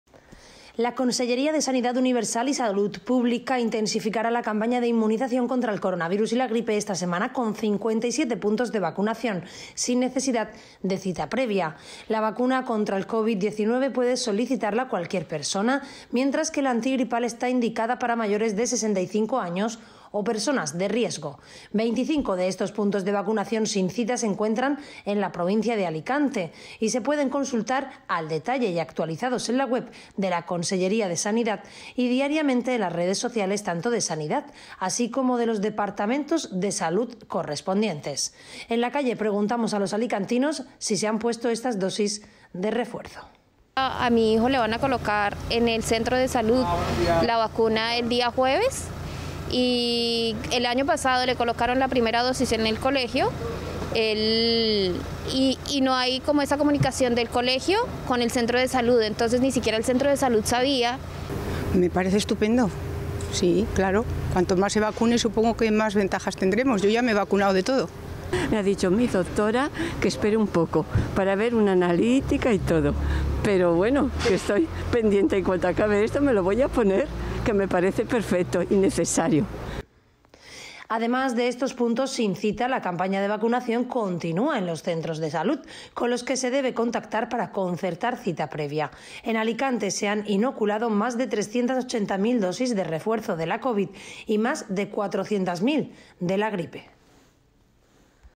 Sanidad abre 57 puntos de vacunación sin cita para coronavirus y gripe
La Conselleria de Sanidad Universal y Salud Pública intensificará la campaña de inmunización contra el coronavirus y la gripe esta semana con 57 puntos de vacunación sin necesidad de cita previa.

La vacuna contra la Covid-19 puede solicitarla cualquier persona, mientras que la antigripal está indicada para mayores de 65 años o personas de riesgo.

25 de estos puntos de vacunación sin cita se encuentran en la provincia de Alicante y se pueden consultar al detalle y actualizados en la web de la Conselleria de Sanidad y diariamente en las redes sociales tanto de Sanidad, así como de los departamentos de salud correspondientes.

En la calle, preguntamos a los alicantinos si se han puesto estas dosis de refuerzo:

Además de en estos puntos sin cita, la campaña de vacunación continúa en los centros de salud, con los que se debe contactar para concertar cita previa.

En Alicante se han inoculado más de 380 mil dosis de refuerzo de la COVID y más de 400 mil de la gripe.